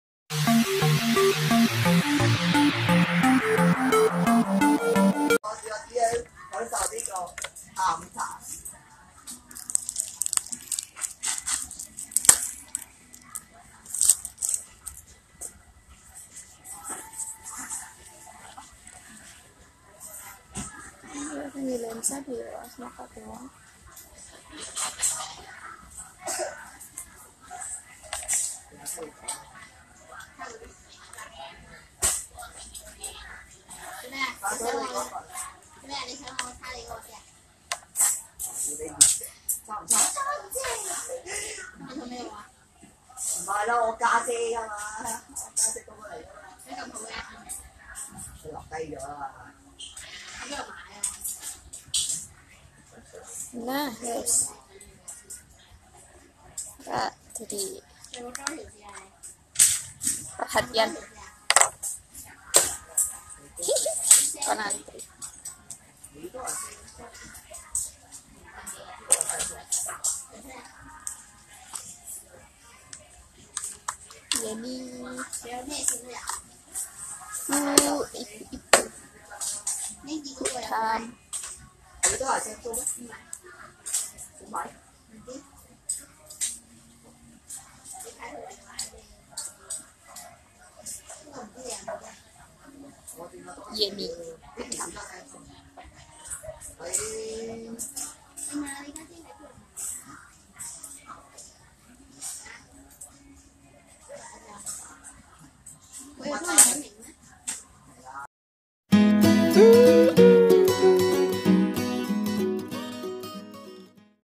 CARA MENCOPOT SOFLENS | ketika diluar rumah
Pas di dalam rumah nih keliatan bagus, saat di luar rumah dipakek gg cocok apalagi bermakeup tipis, jadi malu diliatin orang orang. nah inilah tutorial cepat copot lensa disaat kepepet .